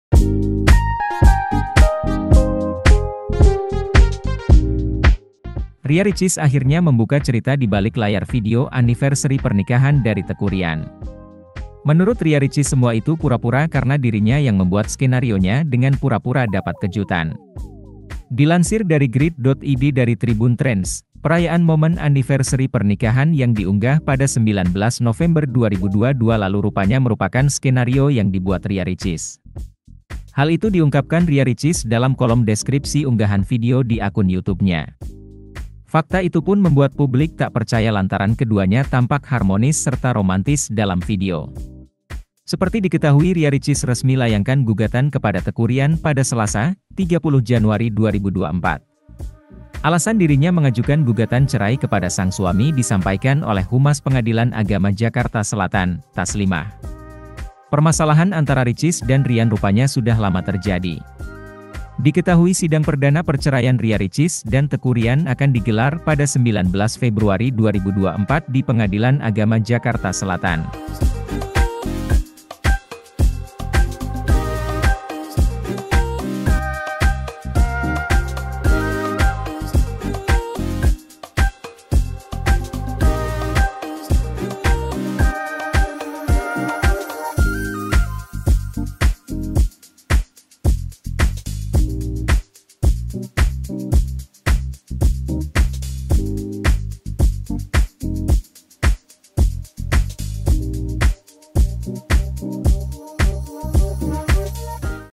Ria Ricis Beberkan Video Kejutan Anniversary Pernikahan Cuma Pura pura
BANGKAPOS.COM - Ria Ricis akhirnya membuka cerita dibalik layar video anniversary pernikahan dari Teuku Ryan.

Menurut Ria Ricis semua itu pura-pura karena dirinya yang membuat skenarionya dengan pura-pura dapat kejutan.

Dilansir dari Grid.id dari Tribun Trends, perayaan momen anniversary pernikahan yang diunggah pada 19 November 2022 lalu rupanya merupakan skenario yang dibuat Ria Ricis.

VP: Andre